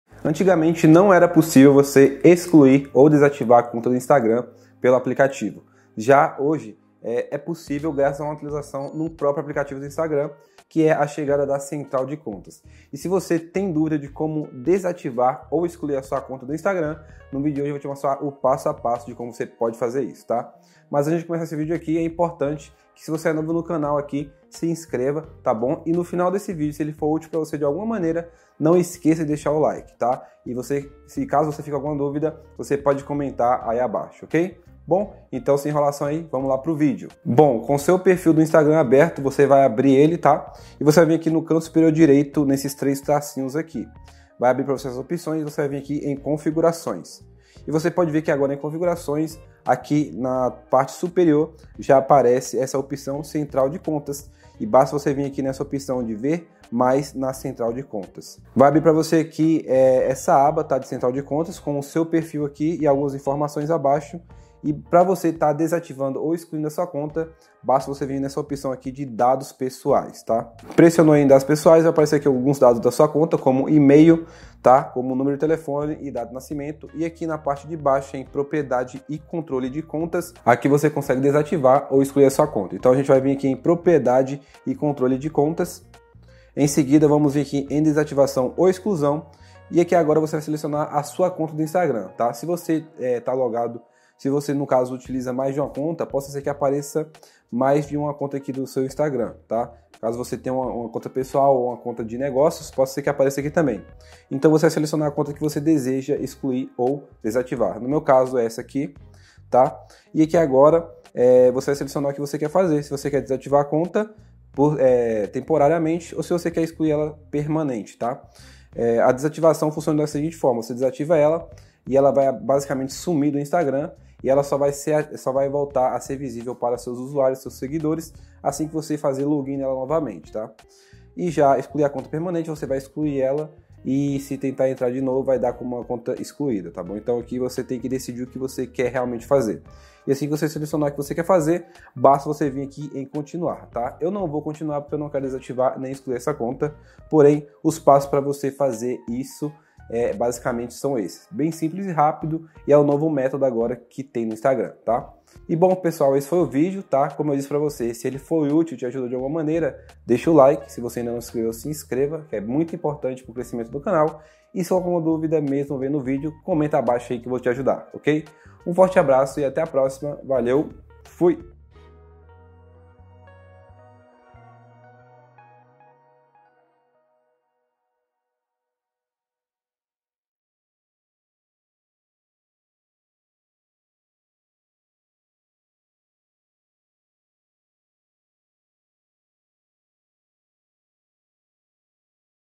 Como EXCLUIR/DESATIVAR CONTA do INSTAGRAM em 2023
Neste tutorial passo a passo, vamos mostrar a você como desativar ou excluir sua conta do Instagram usando o Central de Contas.